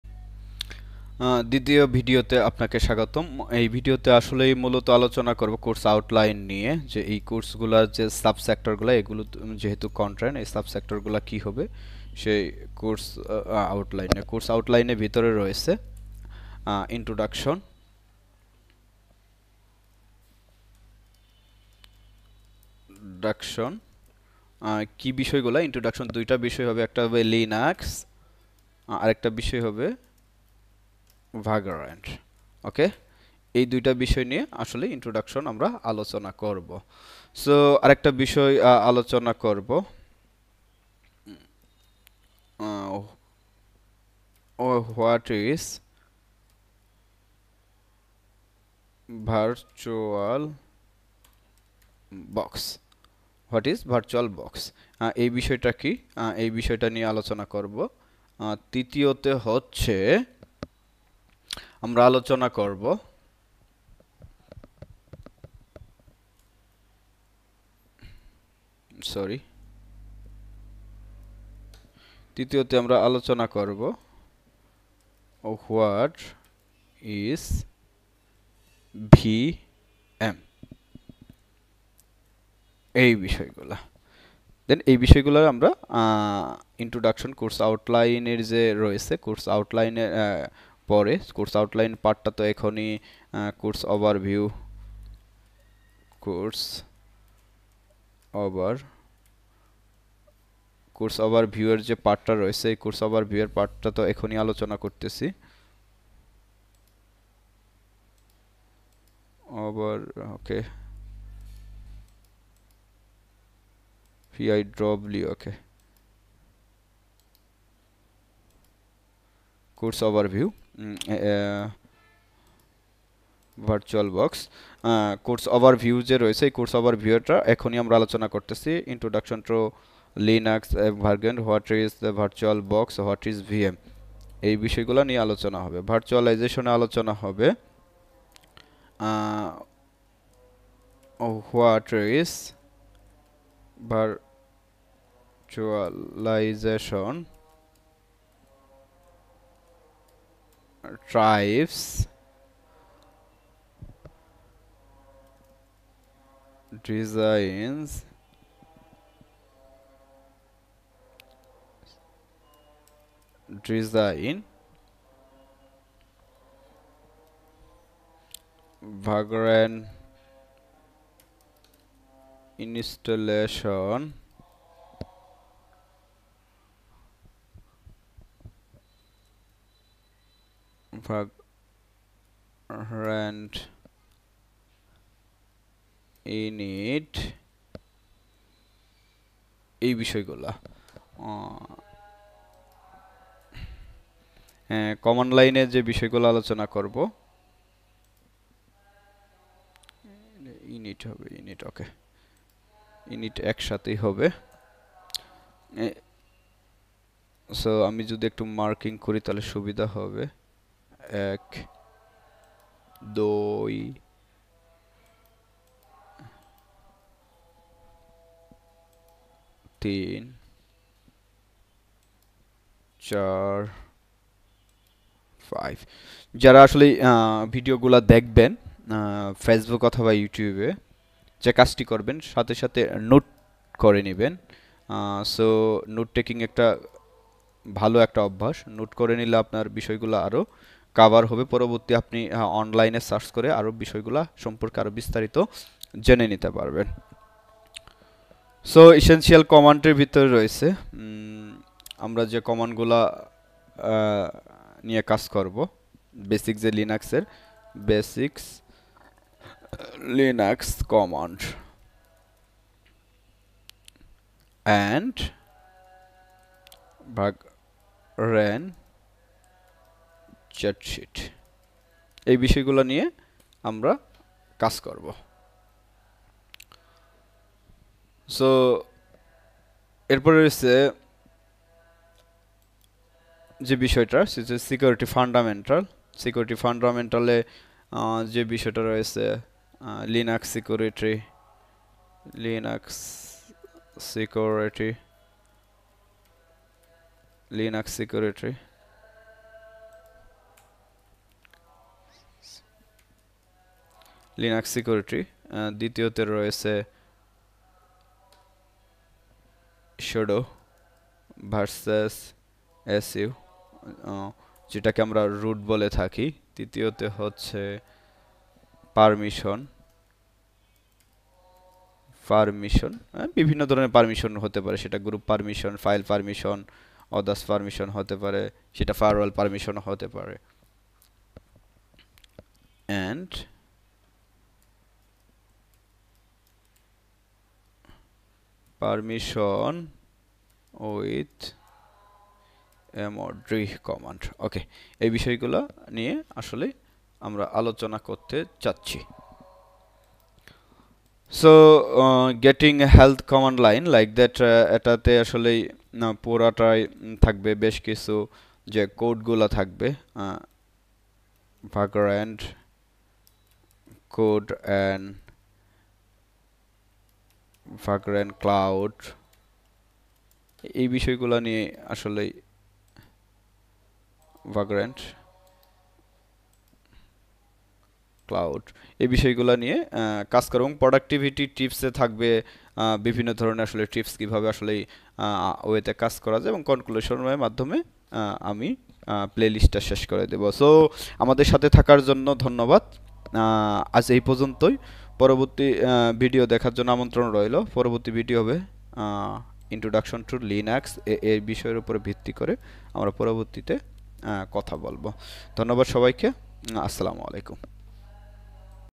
02 : Course Dailts : Linux for Everyone A Hands-on Approach with Vagran
Welcome to Course Details: Linux for Everyone - A Hands-on Approach with Vagrant! In this second episode of our series, we delve deeper into the world of Linux, exploring its functionalities and practical applications. Join us as we navigate through the essentials of Linux operating system, leveraging the power of Vagrant to enhance your learning experience. Whether you're a novice or a seasoned pro, this course promises to equip you with valuable skills to master Linux. Linux Vagrant HandsOnLearning"